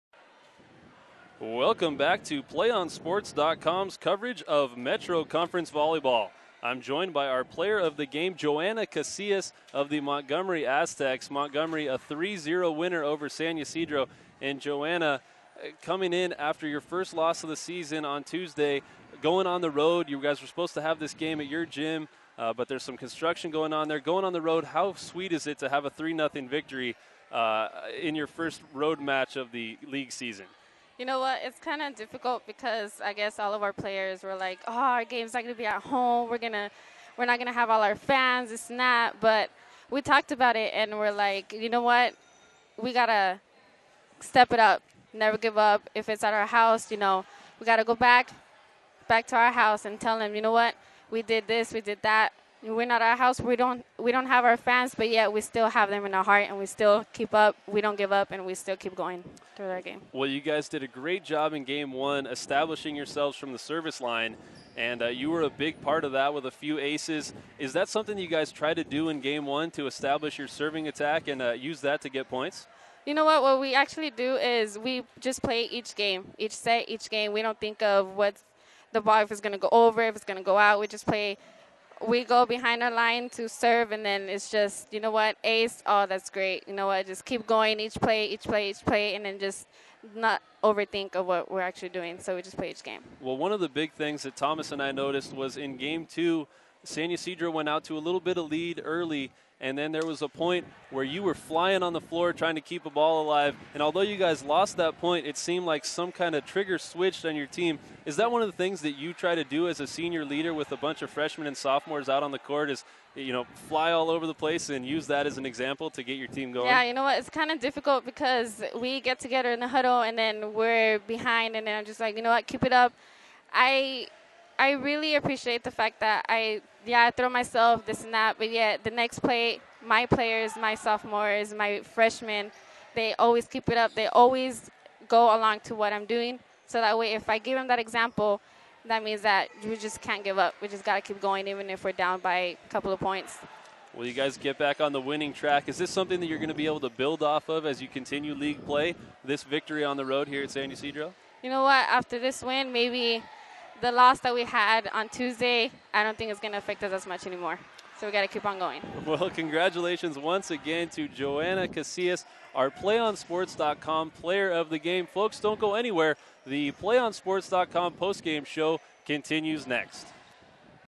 Player of the Game Interview with Montgomery S Joana Casillas
Interview with Joana Casillas after Montgomery defeats San Ysidro 3-0 in this CIF San Diego Section contest.